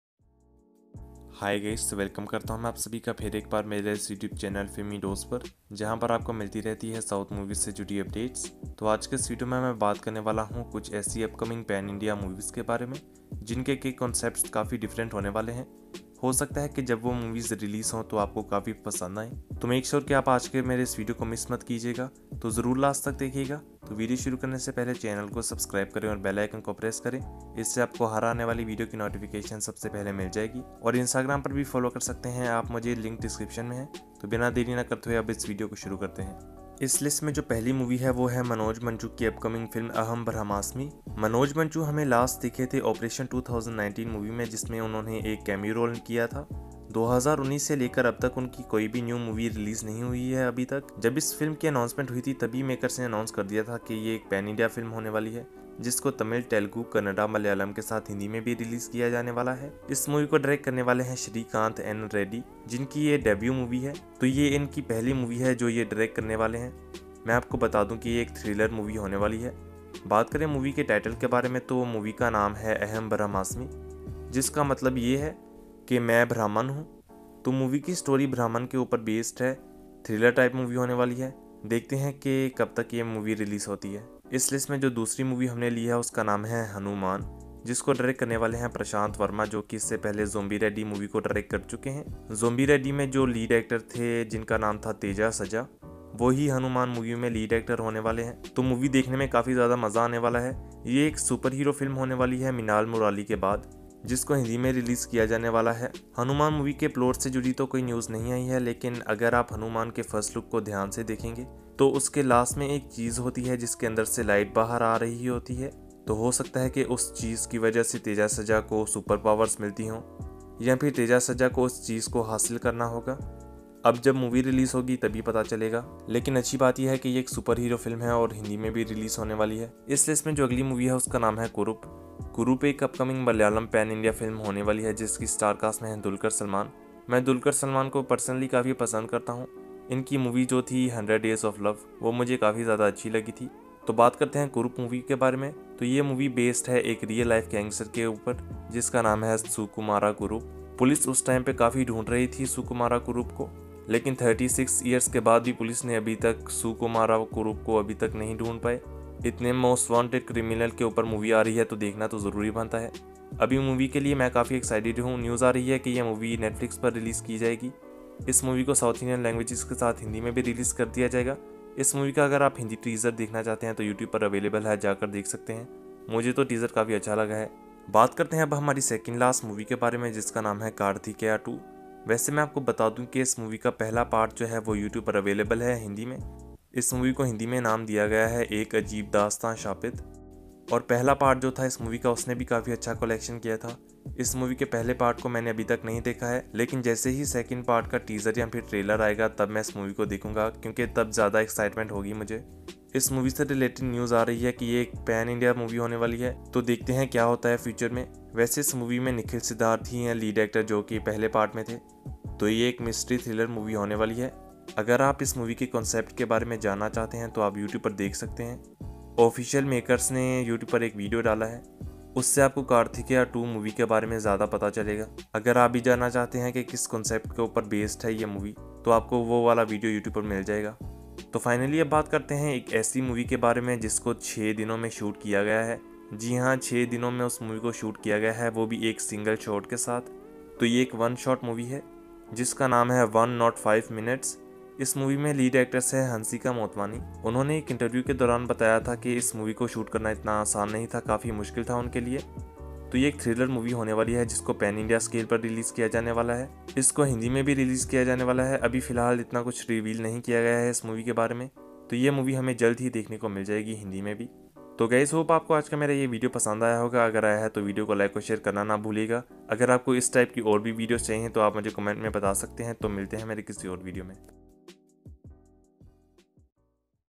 Upcoming PAN INDIA Movies With Interesting Concepts | Filmy Dose
In This Video I Have Talked About Upcoming PAN India Movies With Interesting Concepts. Hope You All Like It 😊